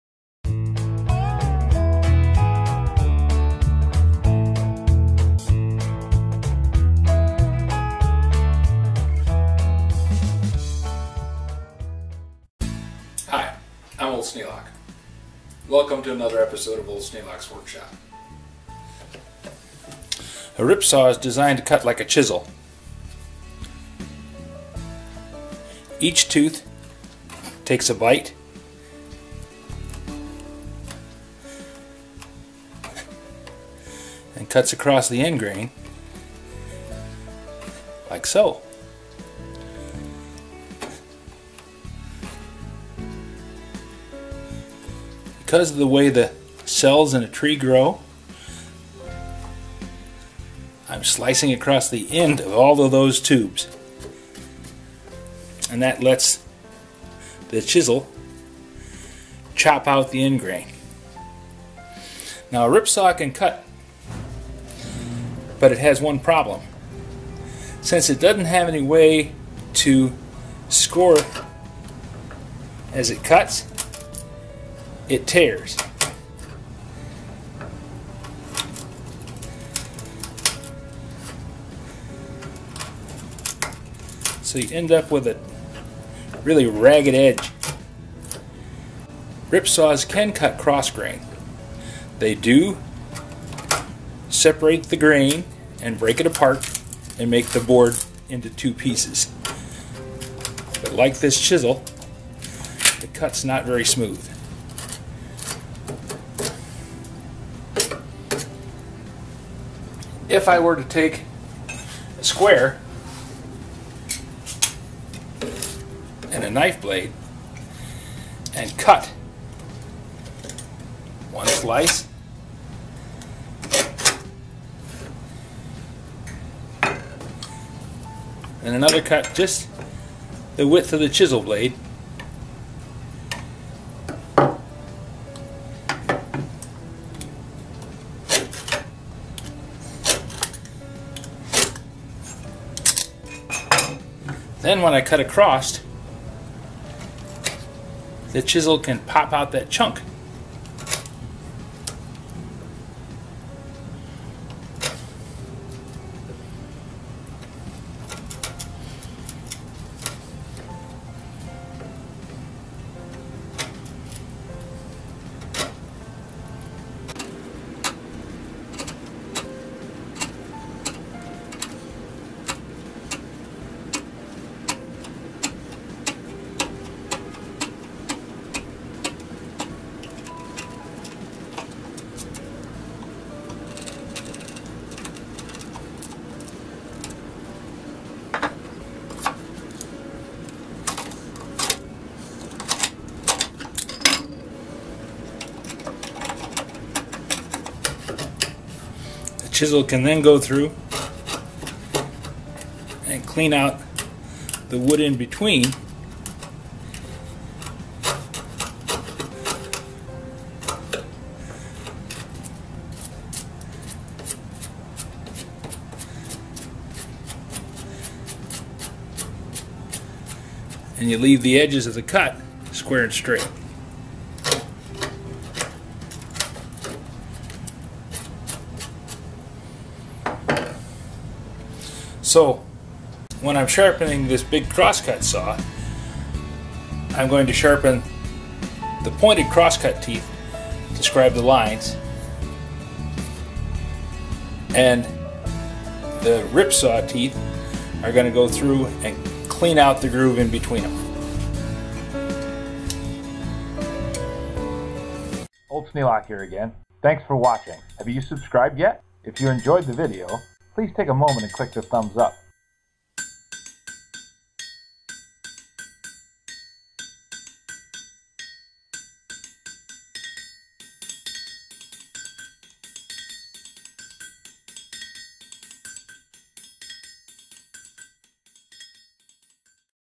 Sharpening A One Man Crosscut - Pt 2 - How It Works - by Old Sneelock's Workshop
There are many types of saws. One Man Crosscut Saws are two saws that only work if the timing is correct
A good overview of the use of manual tools in clearing and maintaining trails is available